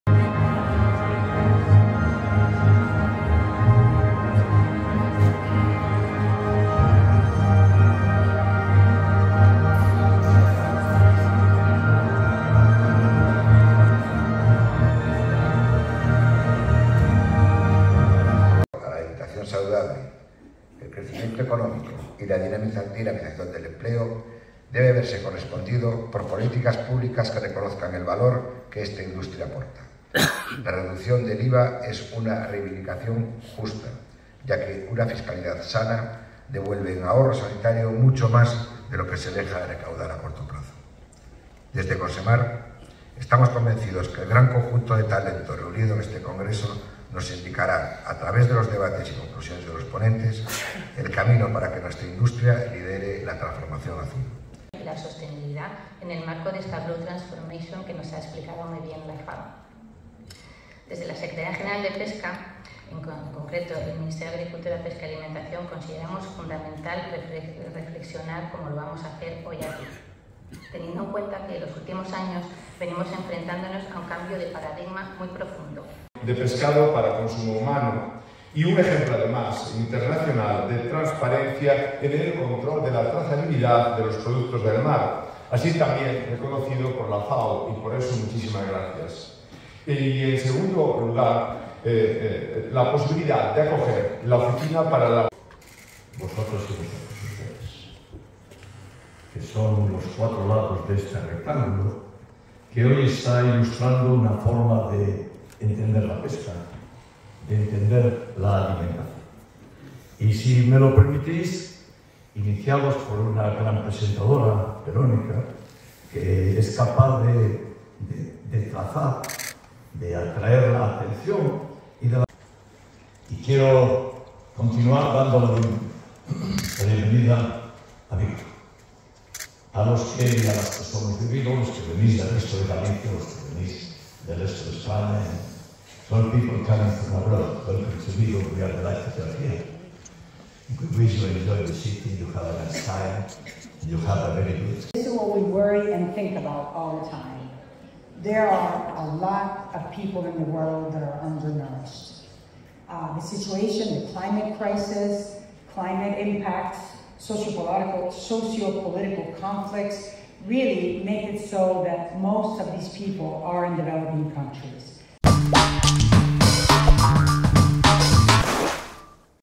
El congreso de la FAO centra su principal objetivo en la transformación azul
📹 Jorge V. Landín